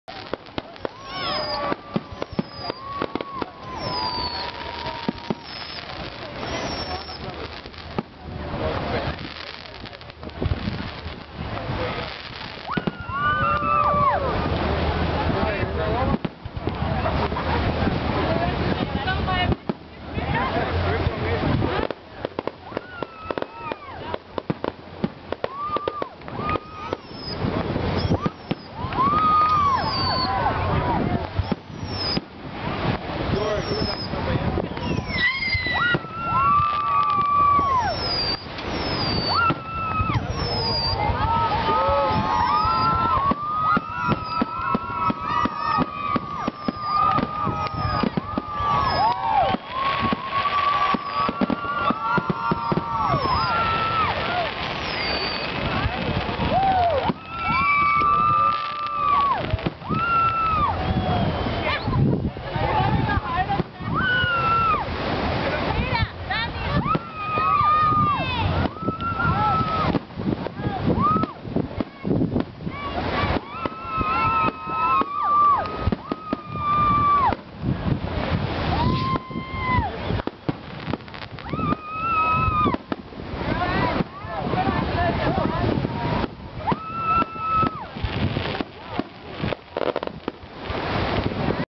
4th of July FIREWORKS Remix This Video
Creative Commons Fireworks that you can Remix on YouTube. Spectacular Miami Beach Fireworks. Miami B-roll presents FREE STOCK FOOTAGE that you can use in your videos and productions